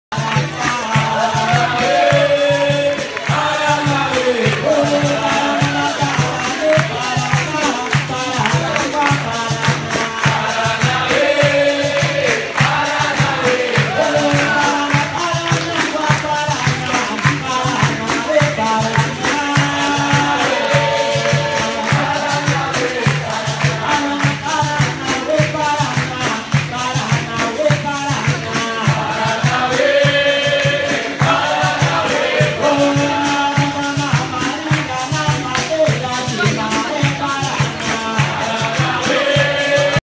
Brasil Capoeira Crianças Bern Schweiz
Grupo Brasil Capoeira Mestre Matias sein Sohn Luan am Capoeira Spiel.